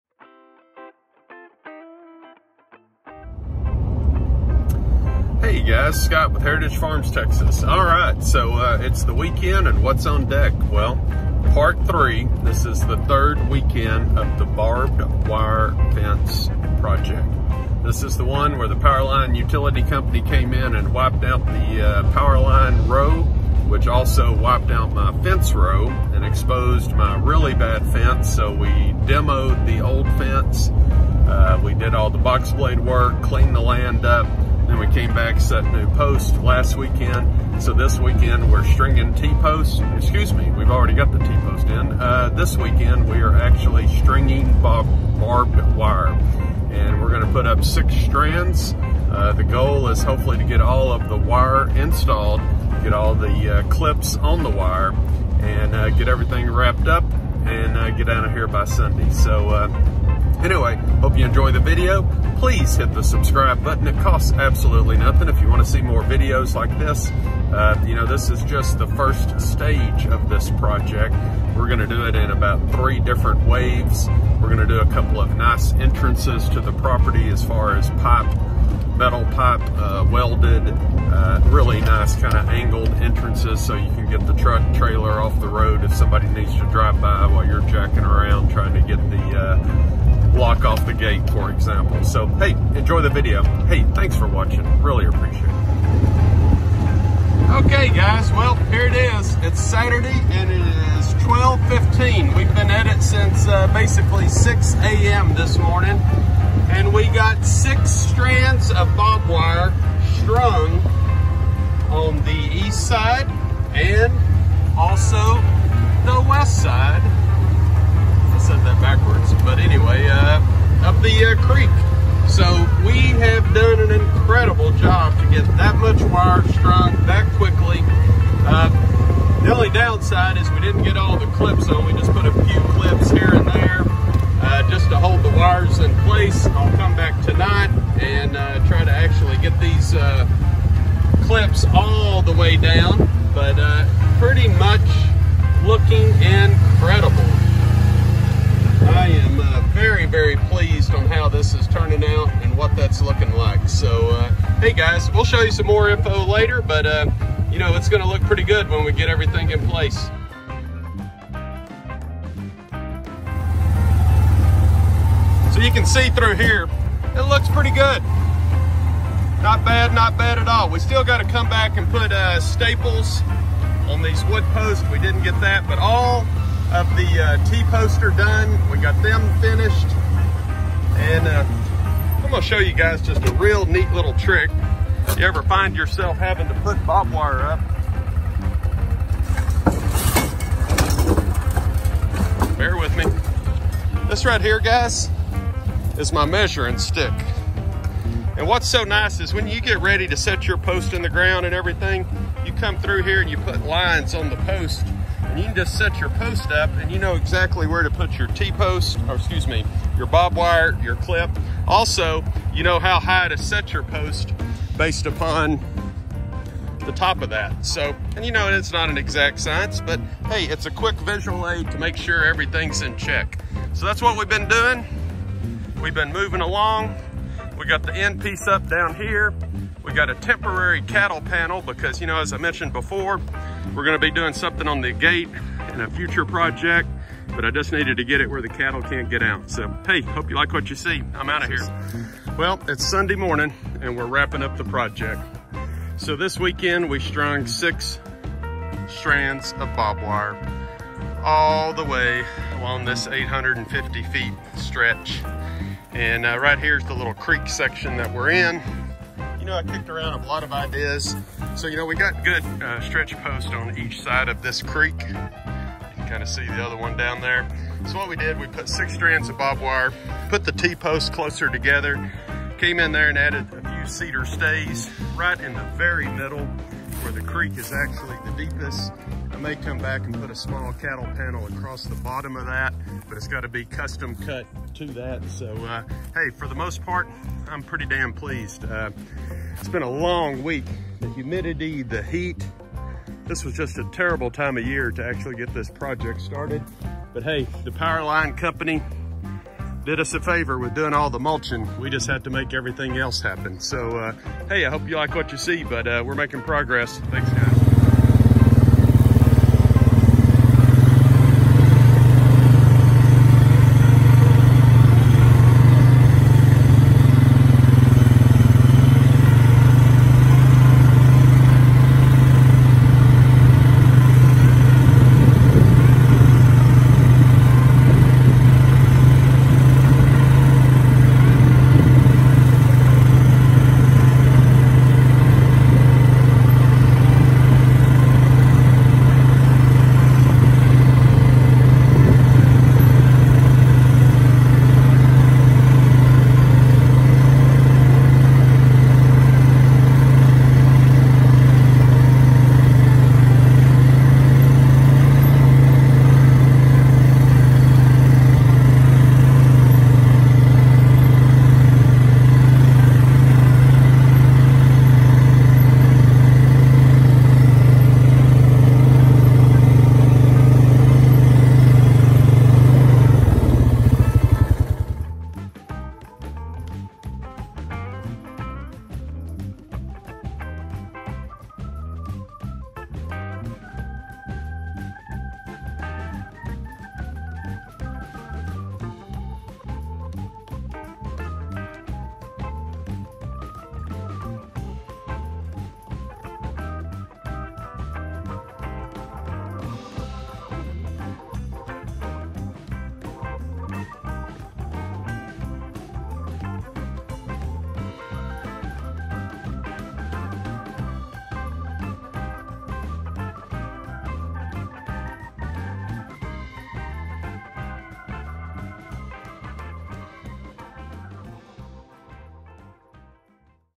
Barbed Wire Fence Project III
Final installment into the construction of 1,000 feet of barbed wire fence.